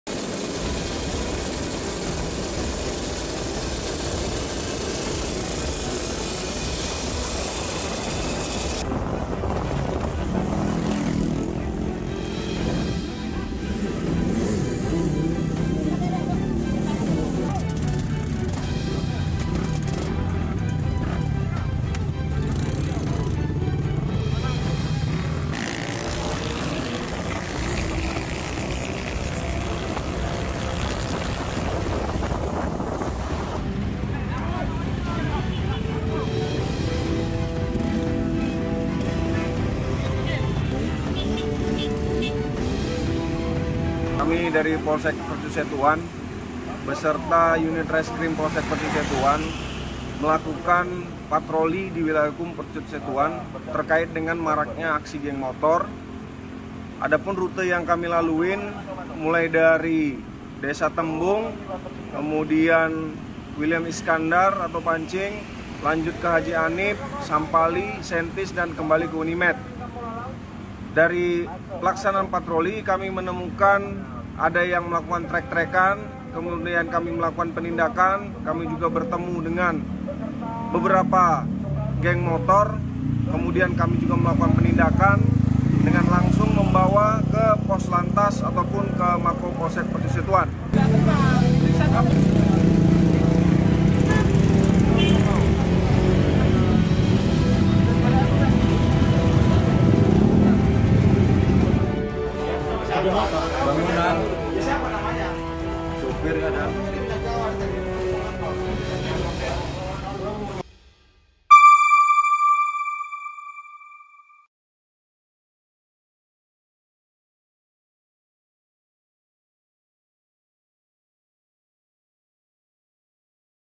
Polisi Berbaju Preman Sergap Geng Motor saat Balap Liar di Medan
Puluhan polisi bermotor berbaju preman ini menggerebek aksi balap liar geng motor.

Android: inews.id/android
iPhone: inews.id/ios